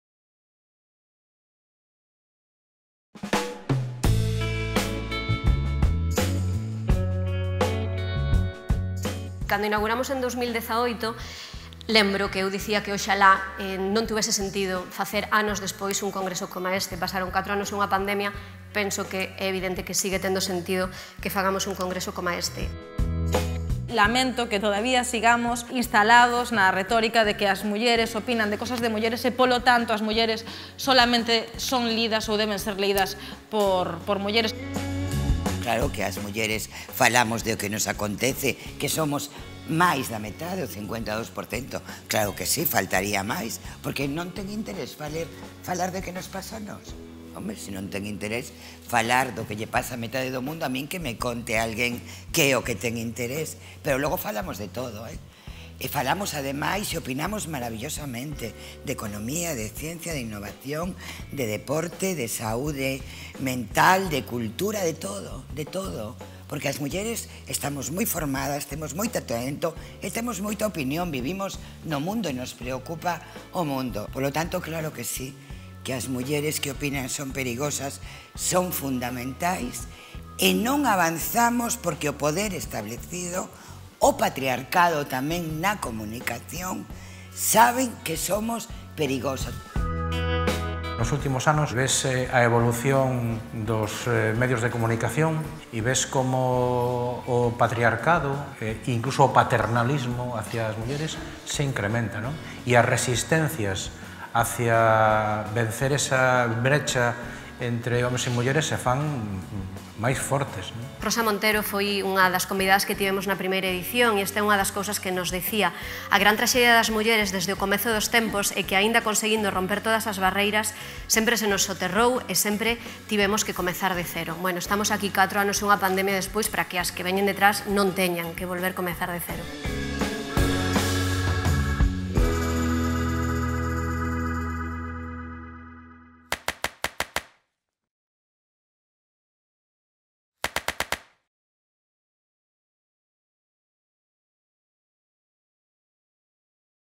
Apertura das xornadas "As mulleres que opinan son perigosas"
A presidenta da Deputación, Carmela Silva, o tenente de alcalde de Pontevedra, Tino Fernández e as xornalistas Diana López e Susana Pedreira abren as xornadas do V Encontro de Mulleres Xornalistas "As mulleres que opinan son perigosas".